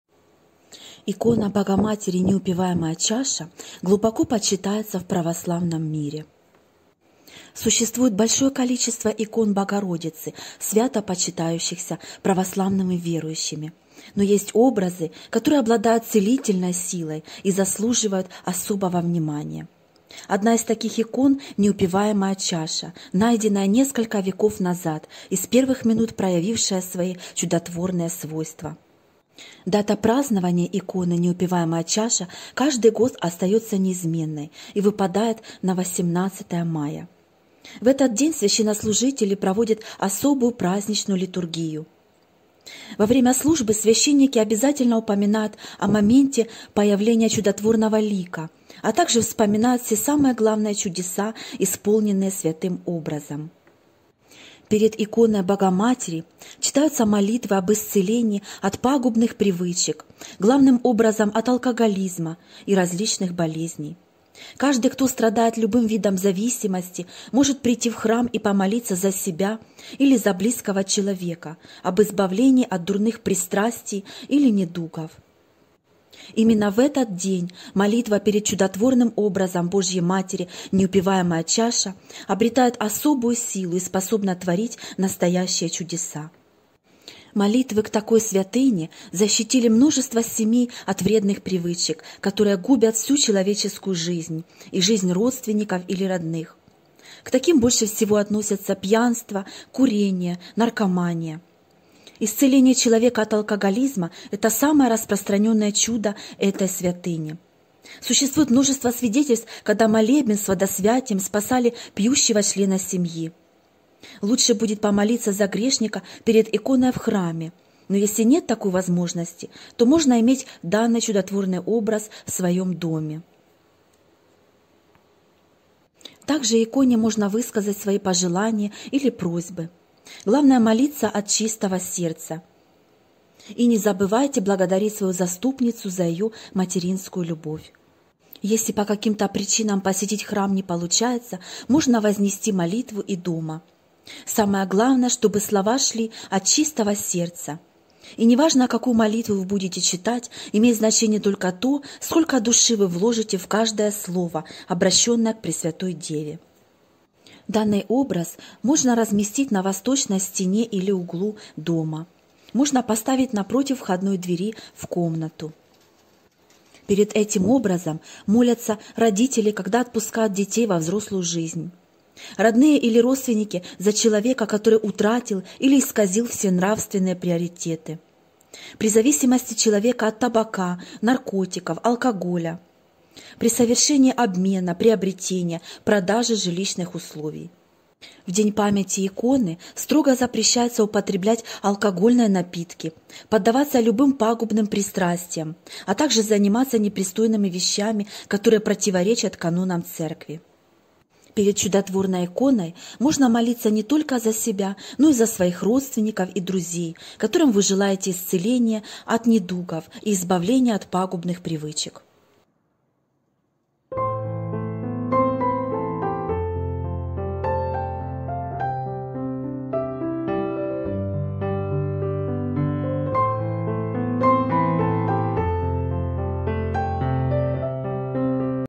18 мая праздник ДЕНЬ ИКОНЫ БОЖИЕЙ МАТЕРИ " НЕУПИВАЕМАЯ ЧАША " . ОТ ЧЕГО ИСЦЕЛЯЕТ...
Пример молитвы к ИКОНЕ БОЖИЕЙ МАТЕРИ " НЕУПИВАЕМАЯ ЧАША " :

«О, великая Владычица Небес! Пресвятая Дева, Матерь Господа! К Тебе обращаюсь в молитве и молю Тебя о прощении. Прости меня, Богородица, за все мои деяния греховные, за все злые слова, за мысли нечистые. Каюсь я и молю о Твоей милости и благословении. Прошу Тебя, Пресвятая Дева, перед образом Твоим: избавь меня от привычек пагубных, от зависимостей и болезней. Пусть в душе моей наступит покой и ничто не терзает меня изнутри. Пусть разум мой очистится, и освобожусь я от оков зла и гибельных пристрастий. Услышь меня, Богородица, и окажи мне Свою помощь и поддержку. Во веки веков. Аминь».